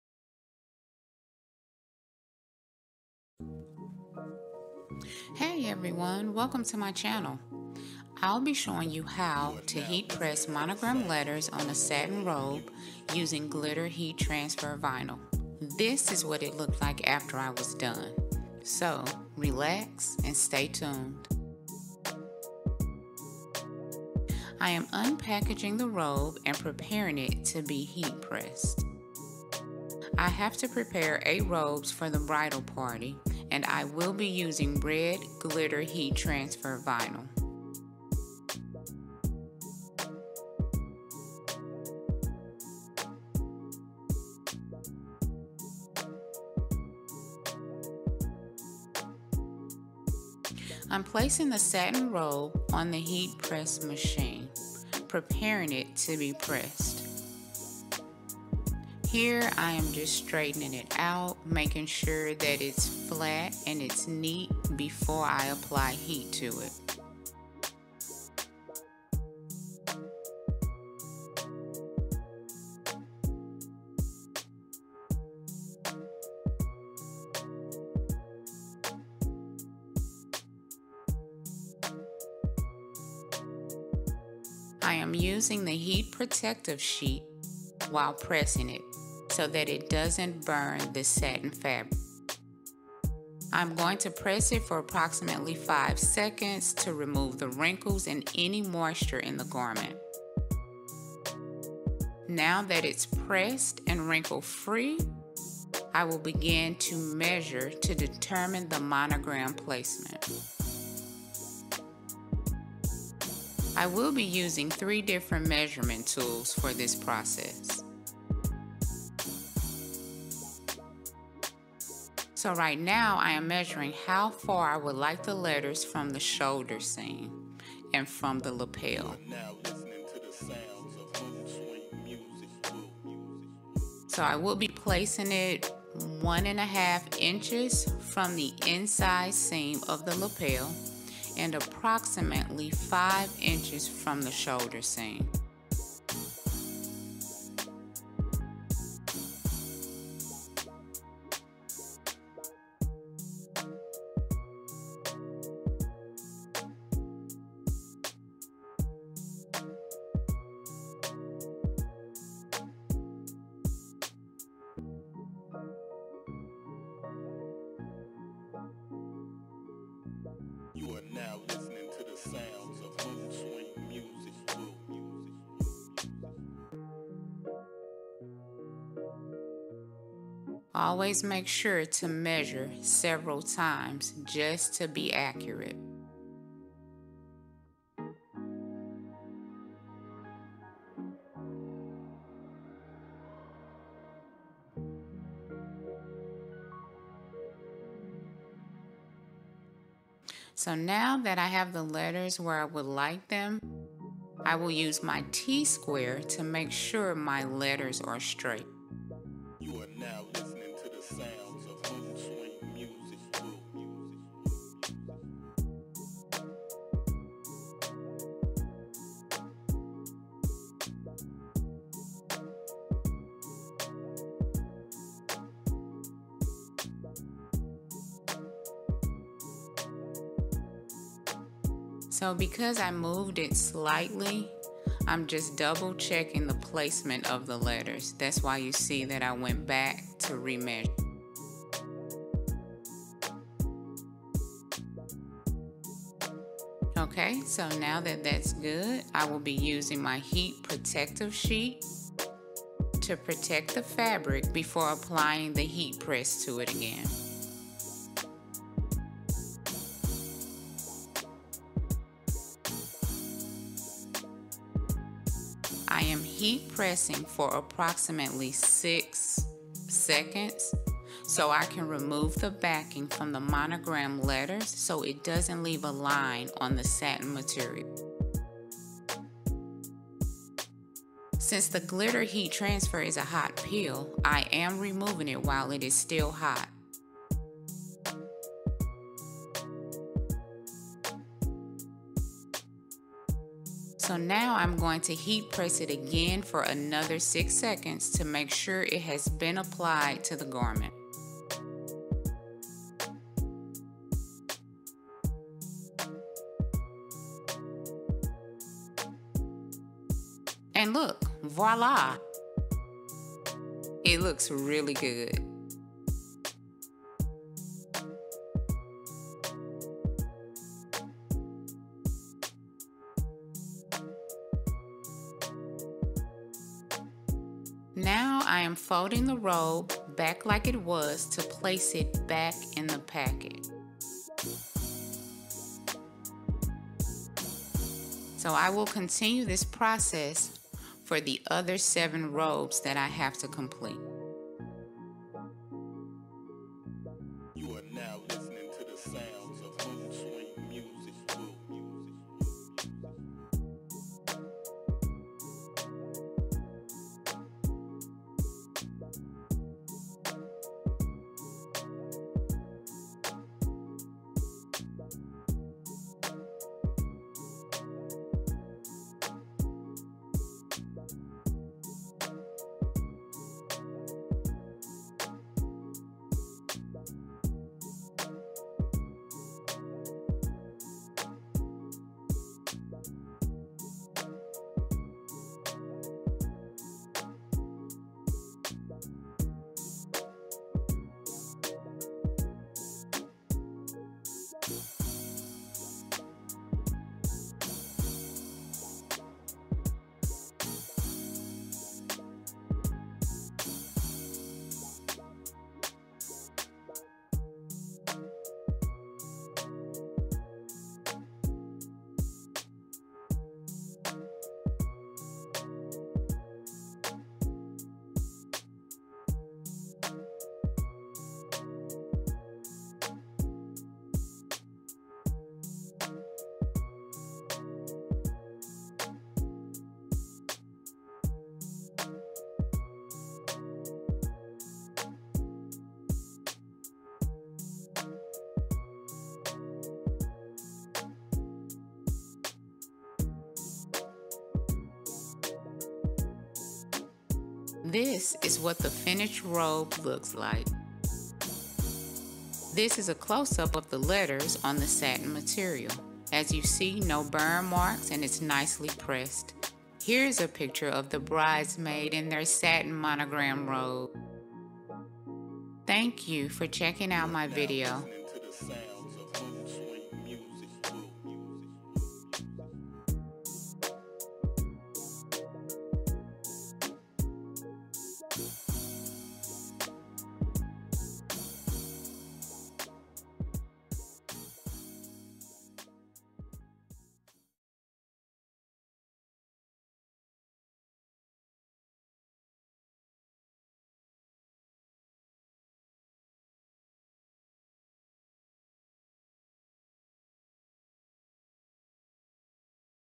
DIY | HEAT PRESS ON SATIN | BRIDESMAIDS ROBES | WEDDING IDEAS
In this video I will share with you a DIY on how I applied heat press glitter on a satin bridesmaids robe.